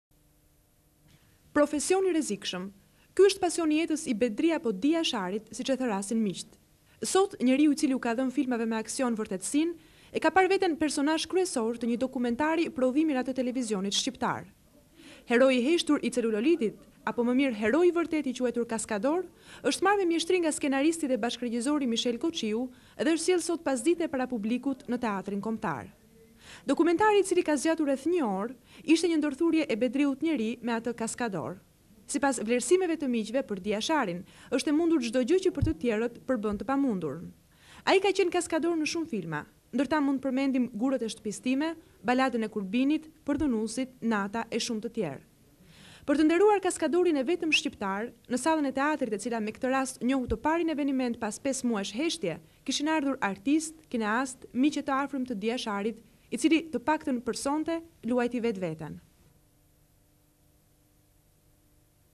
Dokumentari Bedri Jasharit (26 Nentor 1998)
Arkiva - Tv Klan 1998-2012; Pamje nga premiera e dokumentarit te Bedri Jasharit.

Pasioni i jetës i Bedri Jasharit është profesioni i frikshëm. Sot njeriu i cili u ka dhënë filmave me aksion vërtetësinë, e ka parë veten personazh kryesor të një dokumentari të prodhimit të Radio Televizionit Shqiptar. Heroi i heshtur i celuloitit apo me saktë heroi i vërtetë i quajtur "kaskador" është marrë me mjeshtëri nga skenaristi dhe bashkëregjisori Mishel Koçiu dhe është sjellë sot para publikut në Teatrin Kombëtar.
Dokumentari i cili ka zgjatur për rreth një orë ishte një ndërthurje e  Bedriut njeri me atë "kaskador". Sipas vlerësimeve të miqve, për të është e mundur çdo gjë që për të tjerët është e pamundur. Ai ka qënë kaskador në shumë filma ku mund të përmendim "Gurët e shtëpisë sime", "Baladën e Kurbinit", "Nata" e shumë të tjerë.